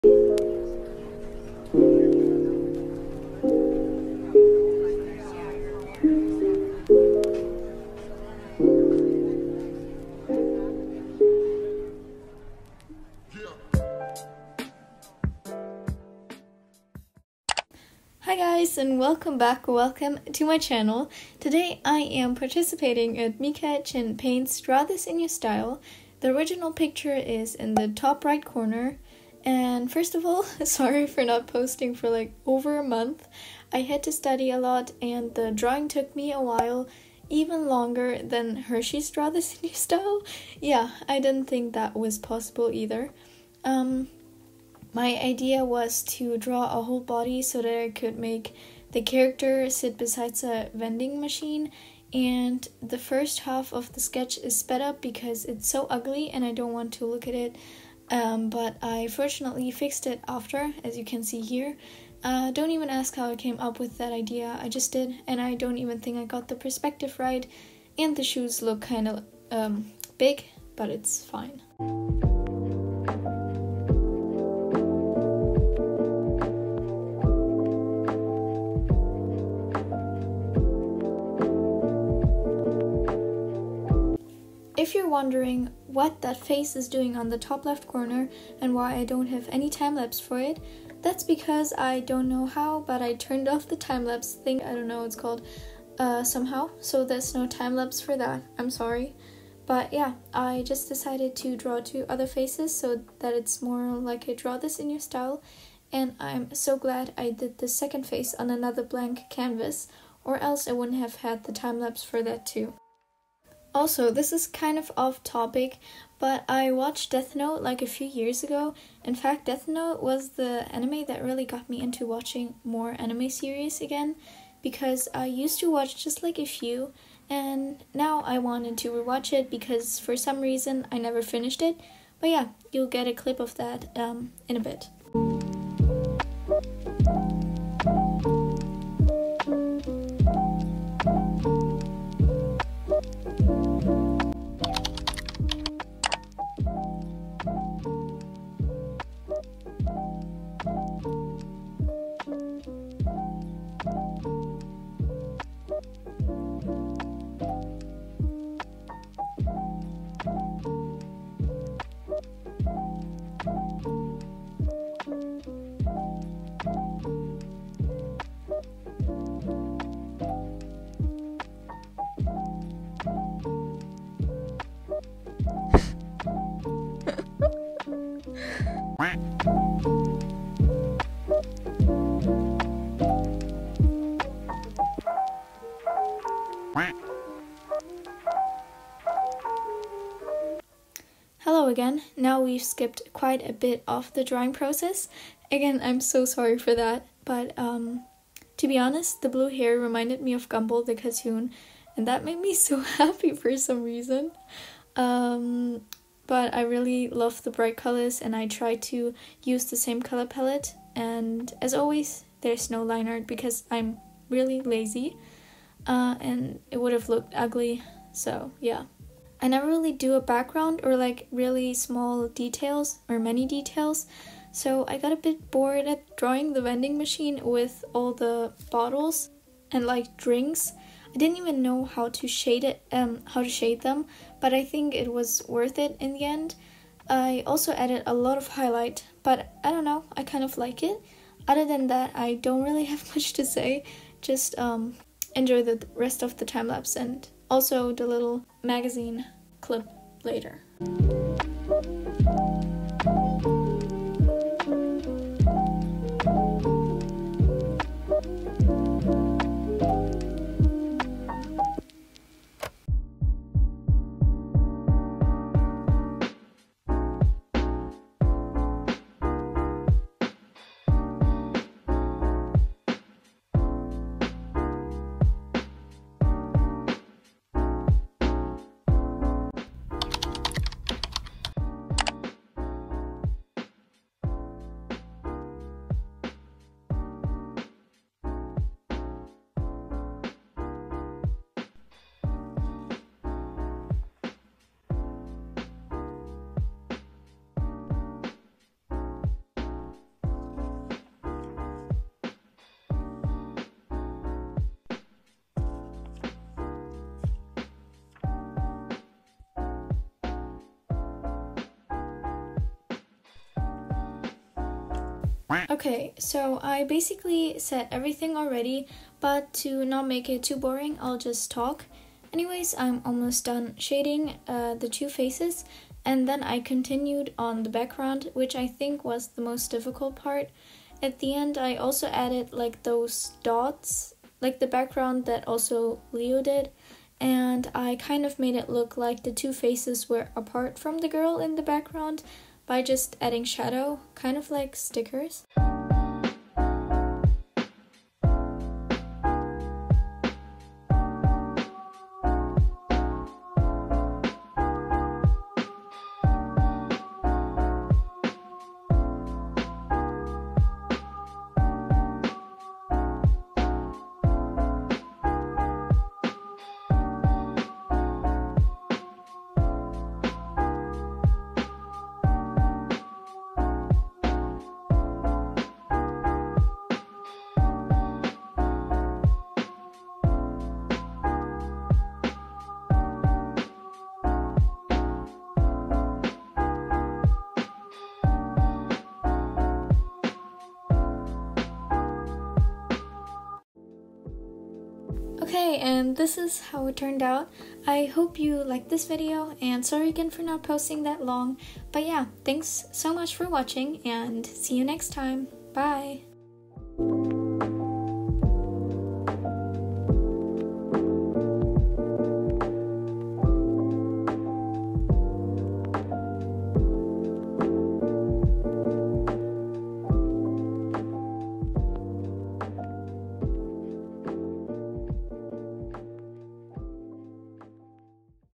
Entry for Mika-chin Paint's DTIYS //
𝗛𝗲𝗹𝗹𝗼 𝗧𝗵𝗲𝗿𝗲! ✨
After a reallyyyy long time I‘m finally posting again and today I‘m participating at Leo’s DTIYS!
𝗧𝗵𝗶𝗻𝗴𝘀 𝗜'𝘃𝗲 𝘂𝘀𝗲𝗱
➪IPad Pro 2019
➪Apple Pencil
➪Procreate

✨I don’t own any music from this video ✨